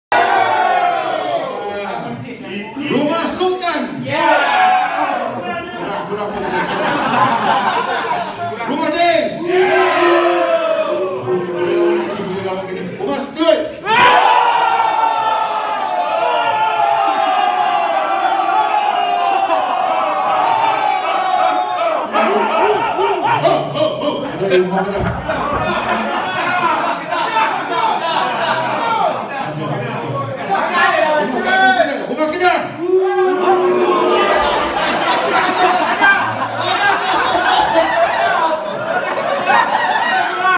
Rumah Sukan KSAH
Rumah Sukan KSAH. Annual Dinner di Cameron Highland Dec 2020.